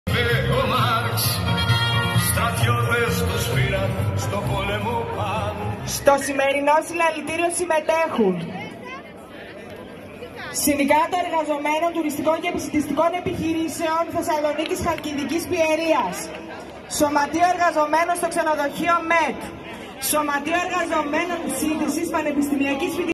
Συγκέντρωση για την ακρίβεια και την πανδημία - GRTimes.gr
Θεσσαλονίκη: Συγκέντρωση και πορεία για την ακρίβεια και την πανδημία στο κέντρο